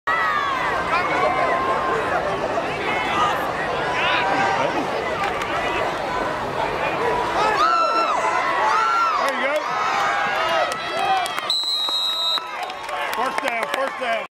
Waynesboro Little Giants versus Stuarts Draft Cougars
Stuarts Draft was victorious over an error-prone Waynesboro team, but there were high points in the game.